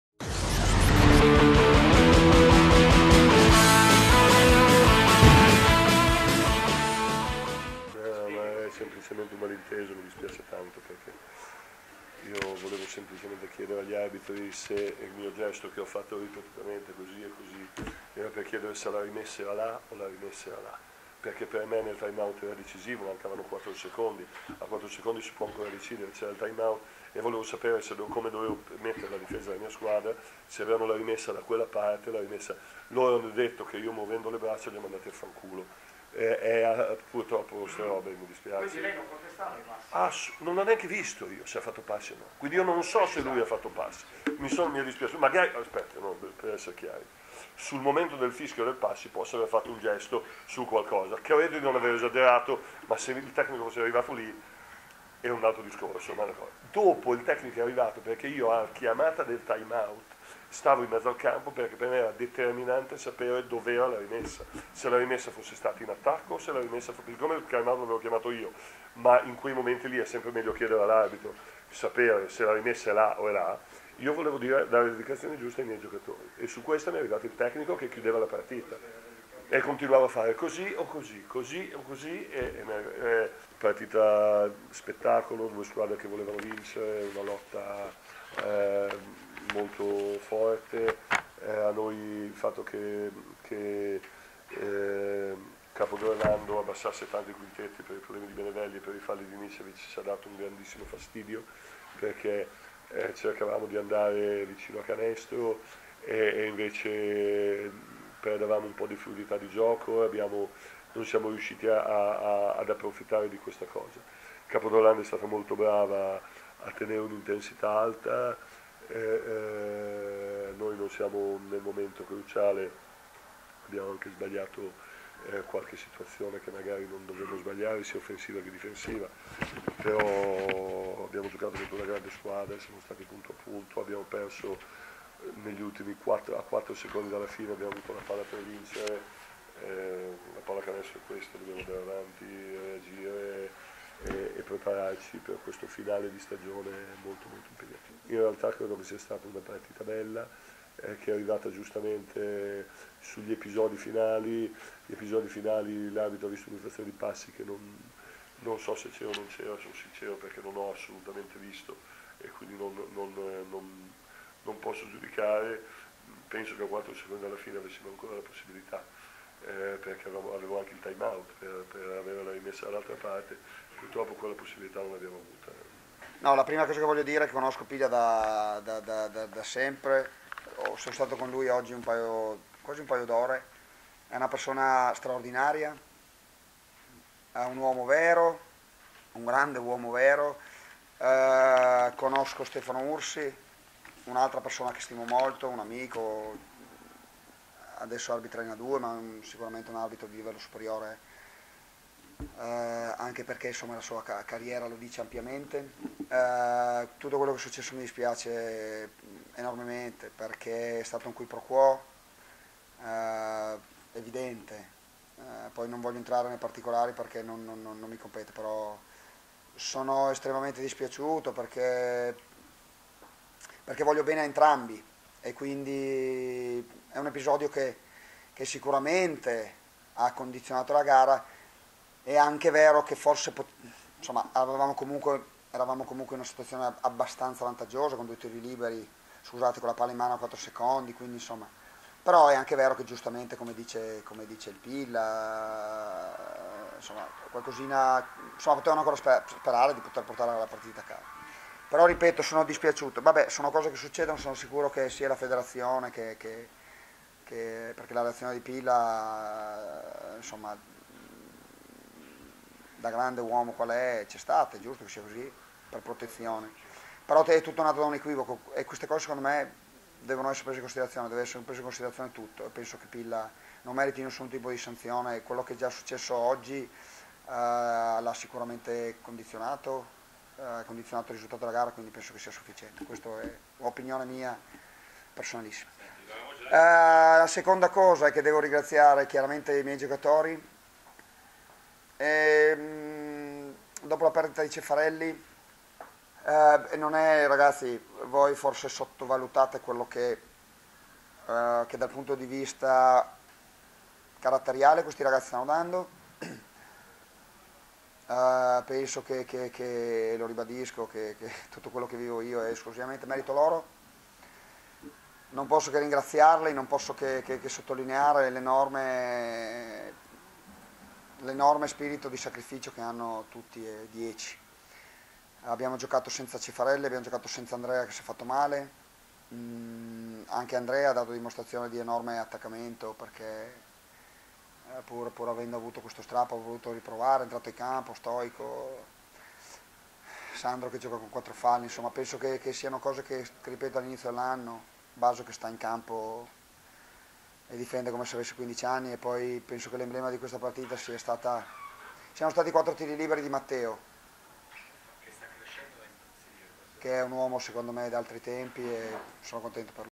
Orlandina-Torino (sala stampa)
Le dichiarazioni al termine del match tra Upea Capo d'Orlando e PMS Manital Torino di coach Pillastrini e coach Pozzecco.

Coach Pillastrini: «Cosa è successo nel finale? Io volevo semplicemente comunicare con gli arbitri perché per me il time-out era decisivo e volevo capire da che parte era la rimessa -- confida ai giornalisti in sala stampa coach Pillastrini -- mentre loro hanno pensato che io li abbia mandati a... Non mi permetto di giudicare l'arbitraggio, è stata una partita spettacolare giocata da due squadre che volevano vincere. Il fatto che Capo abbassasse il quintetto ci ha dato tanto fastidio -- conclude Pillastrini -- Sono stati più lucidi ad approfittare degli errori. Abbiamo anche avuto l palla per vincere, ma adesso dobbiamo pensare al finale della stagione».

«Conosco Pillastrini e Ursi da sempre e sono due persone straordinarie -- dichiara coach Pozzecco nella conferenza post gara -- tutto quello che è successo nel finale, è stato solo un equivoco. Sono dispiaciuto perché voglio bene a entrambi e sicuramente quell'episodio finale ha condizionato la gara. Avevamo, comunque, palla in mano a 4" dalla fine, anche se la gara era ancora in bilico. Sono sicuro che la federazione e Pillastrini chiariranno l'equivoco, penso che non meriti nessun tipo di squalifica. Voglio ringraziare i miei giocatori -- conclude Pozzecco -- Dopo la partenza di Cefarelli, ancora una volta i miei hanno dimostrato di avere lo spirito giusto».